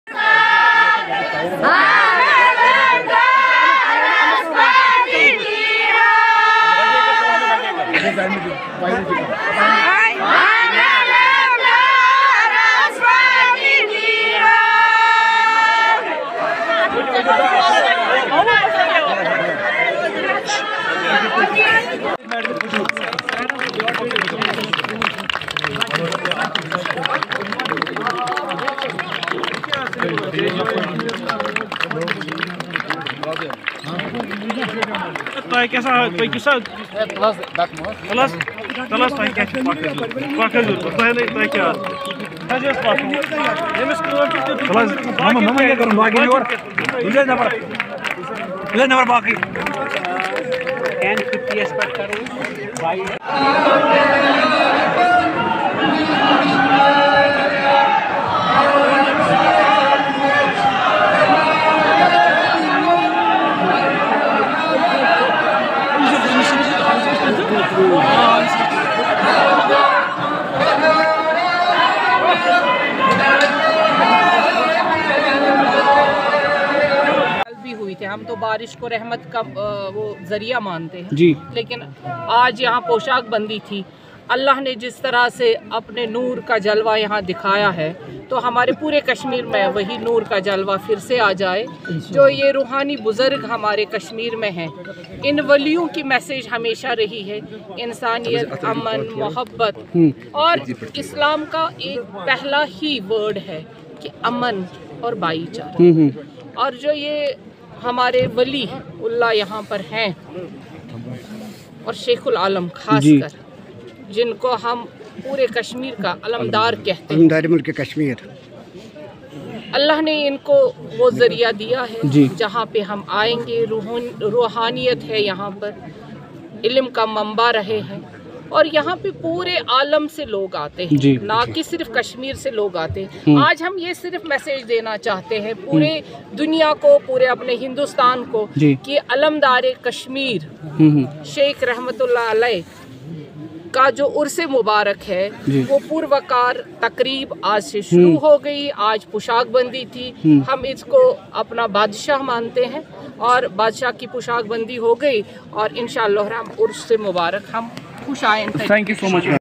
Poshak Bandi' at Hazrat Sheikh-ul-Alam (RA) shrine in Charar-e-sharief; Dr Andrabi visits shrine.
Poshak Bandi' at Hazrat Sheikh-ul-Alam (RA) shrine in Charar-e-sharief: WAQF board chairperson Dr Darakhshan Andrabi visits shrine.